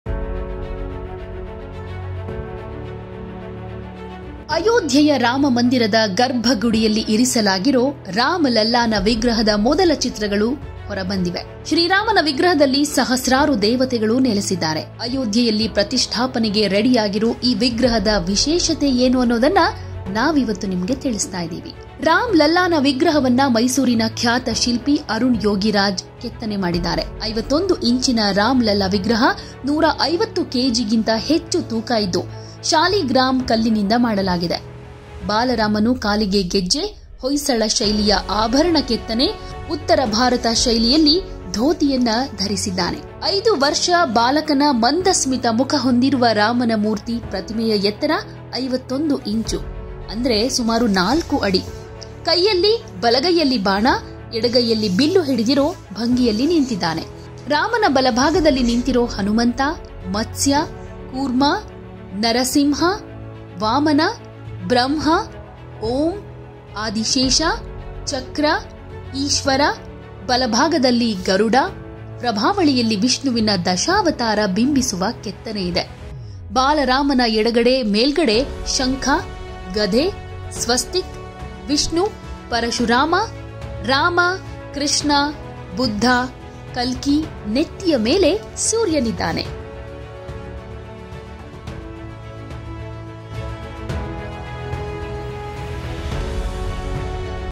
First look of Ram Lalla: ಮಂದಸ್ಮಿತ ಮುಖದ ರಾಮಲಲ್ಲಾ ಮೂರ್ತಿಯ ವಿಶೇಷತೆ ಏನು? | Karnataka Sculptor Created
ಅಯೋಧ್ಯೆ ರಾಮ ಮಂದಿರದ ಗರ್ಭಗೃಹದಲ್ಲಿ
ಇರಿಸಲಾದ ರಾಮ್ ಲಲ್ಲಾನ ವಿಗ್ರಹದ ಮೊದಲ ಚಿತ್ರಗಳು ಹೊರಬಂದಿವೆ. ಶ್ರೀರಾಮನ ವಿಗ್ರಹದಲ್ಲಿ ಸಹಸ್ರಾರು ದೇವತೆಗಳು ನೆಲೆಸಿದ್ದಾರೆ. ಅಯೋಧ್ಯೆಯಲ್ಲಿ ಪ್ರತಿಷ್ಠಾಪನೆಗೆ ಸಿದ್ದವಾಗಿರುವ ಈ ವಿಗ್ರಹದ ವಿಶೇಷತೆಯನ್ನು ತಿಳಿದುಕೊಳ್ಳೋಣ..